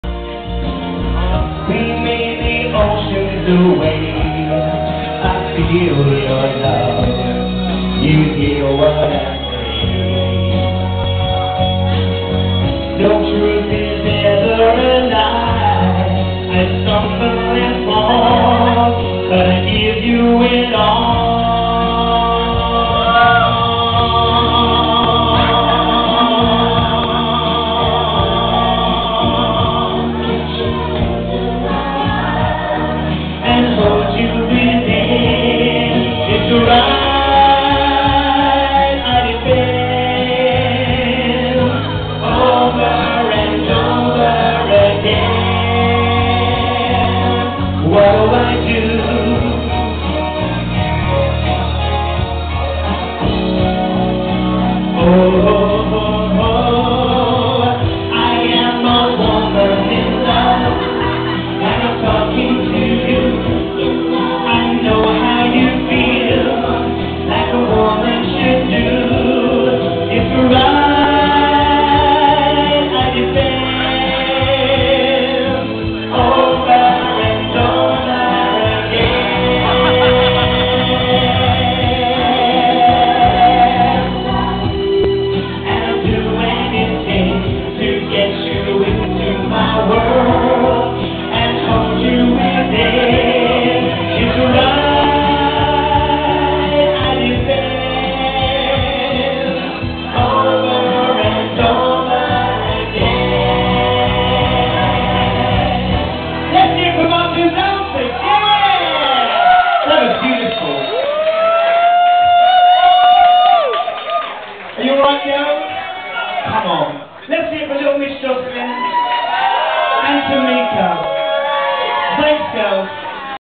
Tameka Empson & Little Miss Jocelyn Dancing With Drag in 2 Brewers! 2010
Actress Tameka Empson who plays Kim in Eastenders & comedienne Little Miss Jocelyn were at my local gay bar, 2 Brewers, having a right laugh & fooling about behind the drag queen!